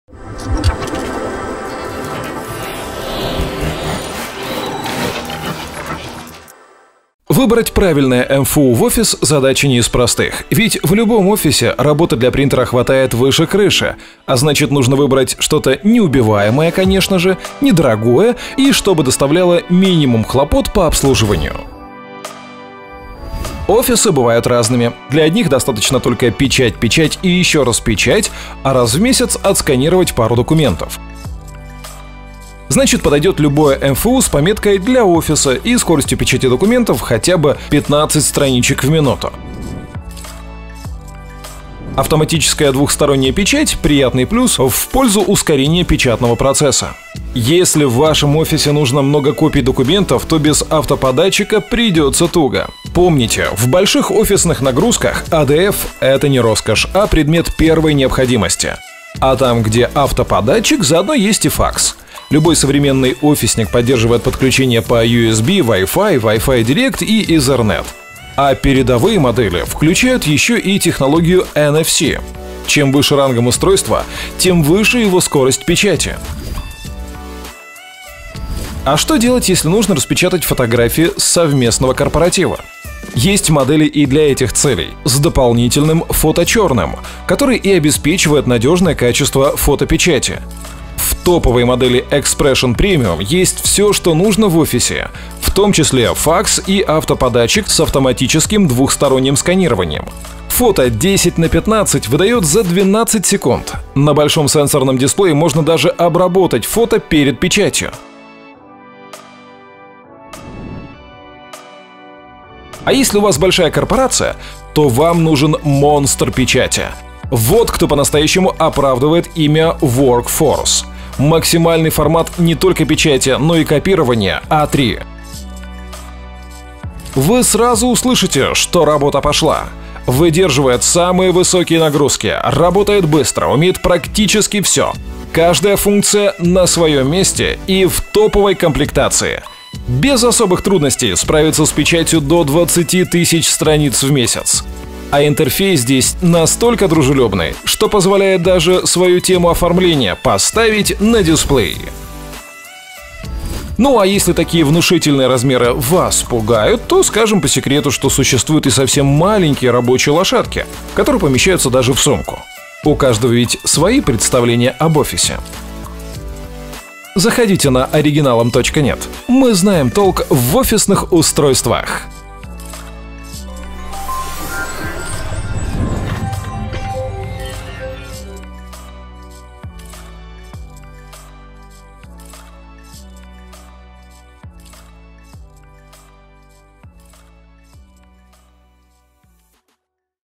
💡 Как выбрать принтер / МФУ для офиса?
Лучшие "монстры" печати в офис:
Выбрать МФУ в офис - задача не из простых. Нужно чтобы и экономичный был, и функционал "по последней моде", и цена по карману не била. Разве такие бывают? Давайте разбираться
на что нужно обращать внимание при выборе. Смотрим:)